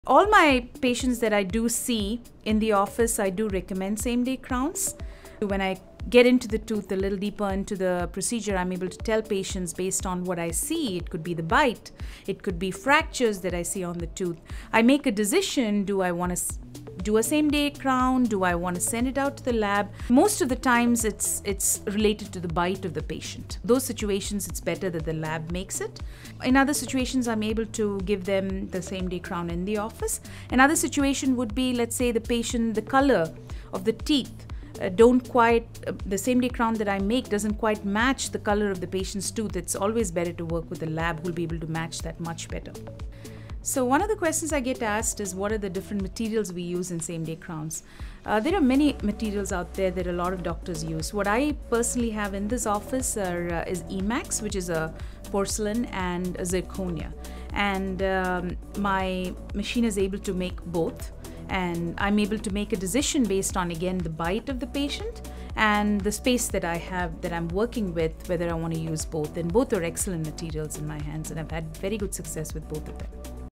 The Crown Chronicles: Same-Day vs. Regular Crowns Explained by Dr. Asha Madhavan
Join Dr. Asha Madhavan in this video as she delves into the world of dental crowns, comparing same-day crowns and regular crowns. Discover the intricacies of different cases where one type of crown is preferred over the other, while Dr. Madhavan emphasizes the importance of prioritizing the health and comfort of the patient. Gain valuable insights into the materials used to craft these crowns, ensuring both durability and aesthetic appeal. This comprehensive discussion sheds light on the advantages and considerations of each crown type, equipping viewers with the knowledge to make informed decisions about their dental care. Experience the expertise of Dr. Asha Madhavan, exclusively on her YouTube channel.

22620 SE 4th St, Suite 230
Sammamish, WA 98074